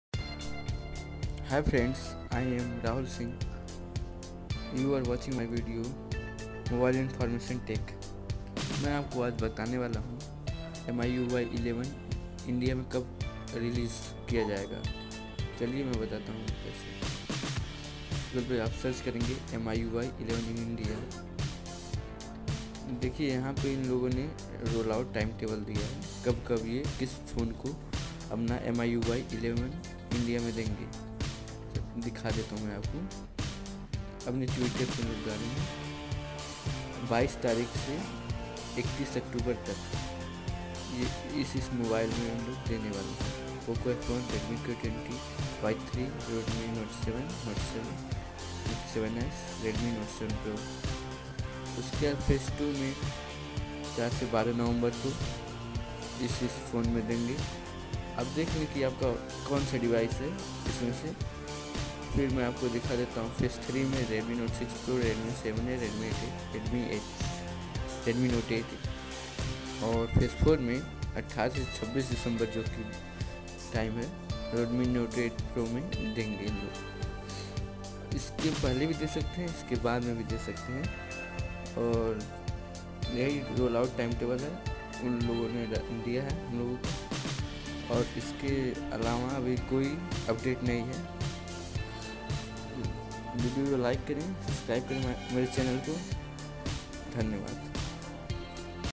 MIUI realising date
I have given the information about the miui 11 . when they are going to release in India.